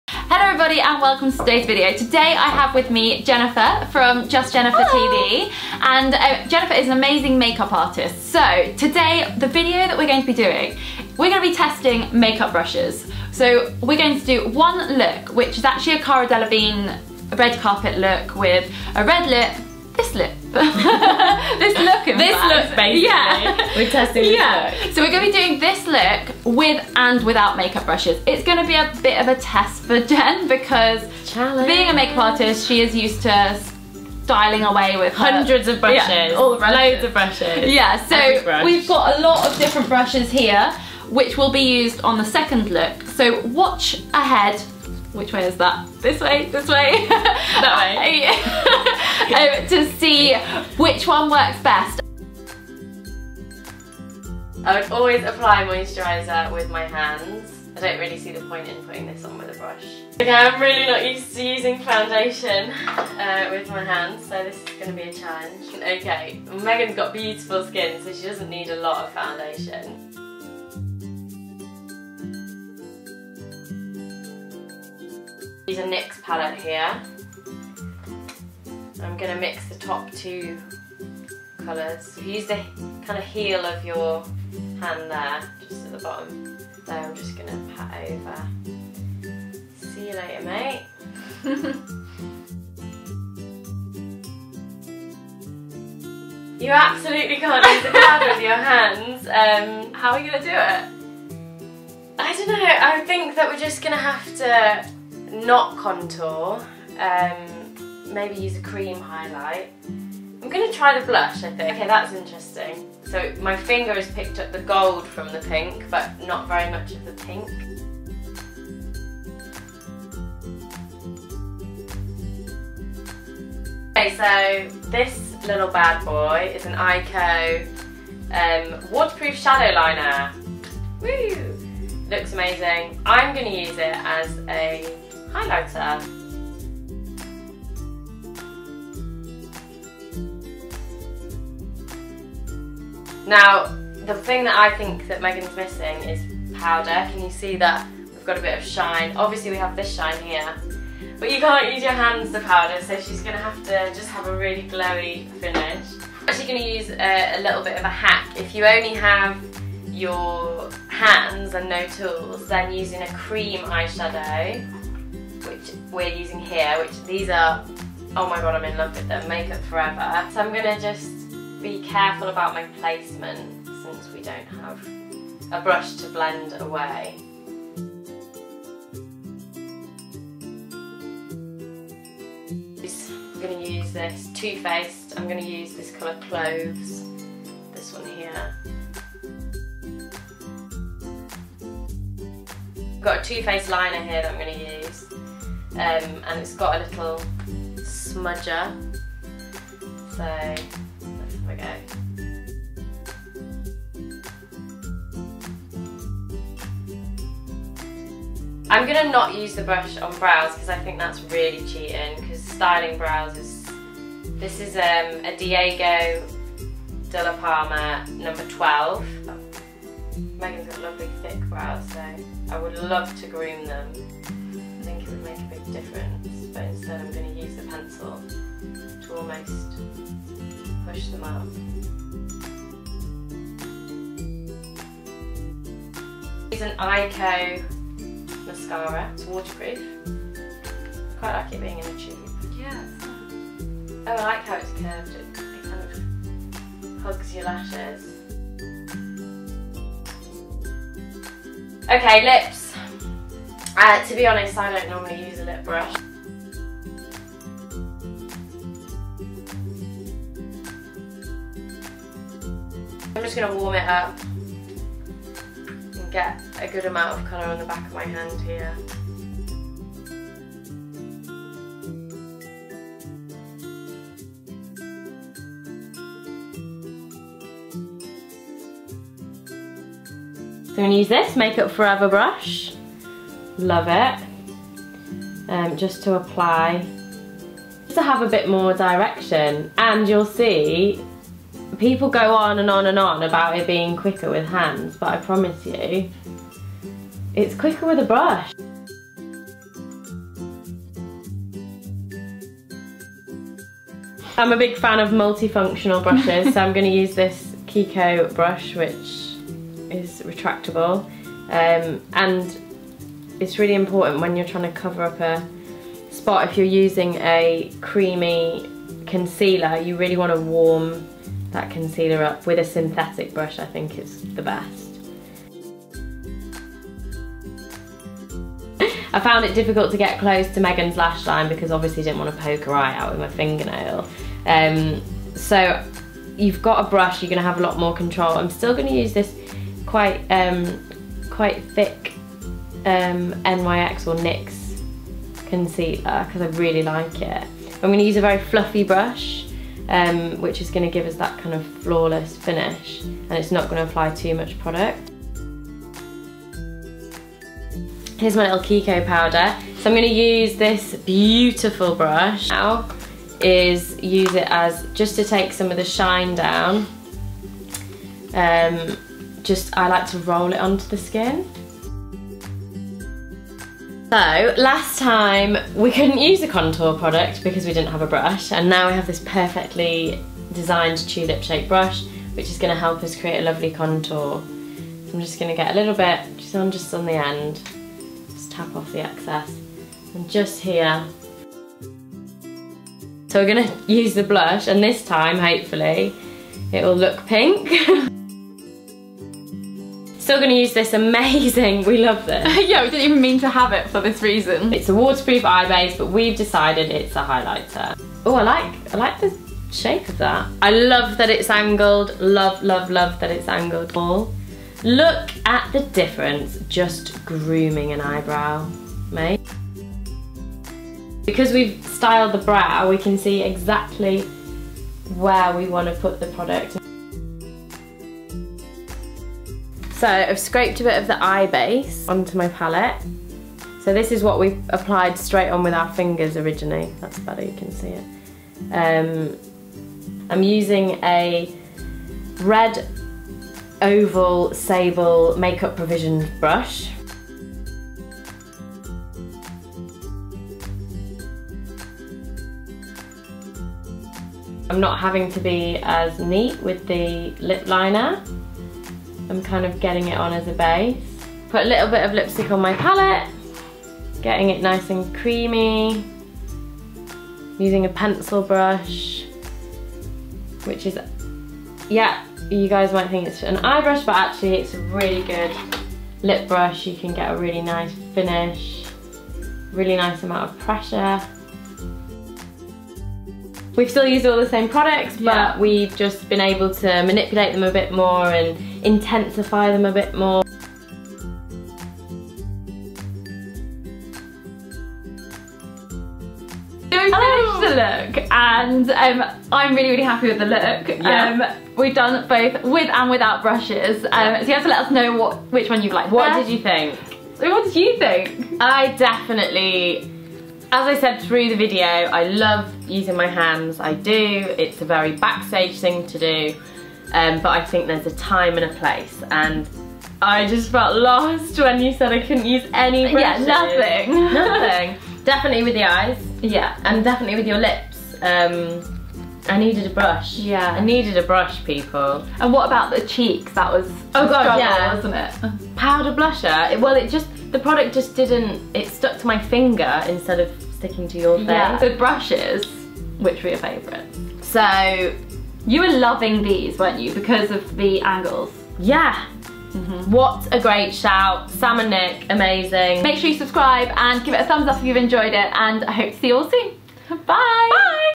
Do We Need Make Up Brushes? | Megan Taylor
FACE
Models Own Scult & Glow - blush and contour (Fair To Light 01)

KIKO lip brush (used as a concealer brush) (Lip Brush 300)